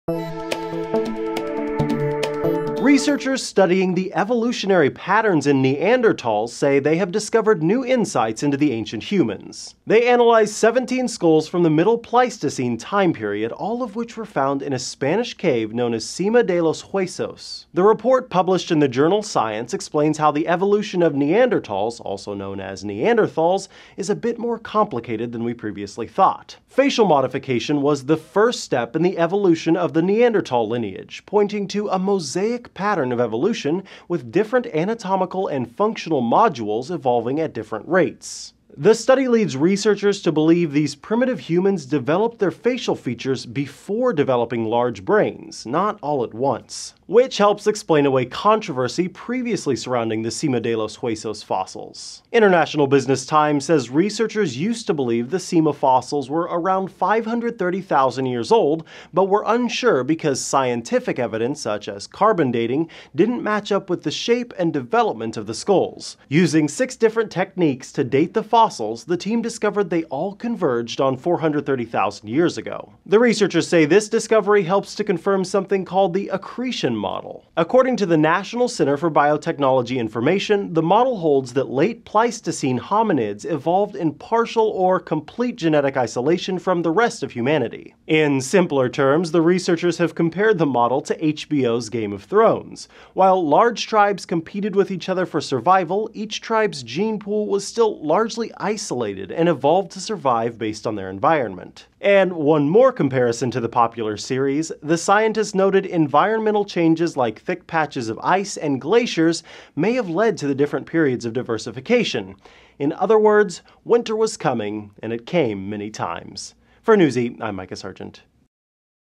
Ancient Skulls Provide Insight Into Neanderthal Ancestry
Researchers studying evolutionary patterns in early Neanderthals have quelled controversy and provided more evidence for the accretion model.

Image via: Flickr / Paolo C.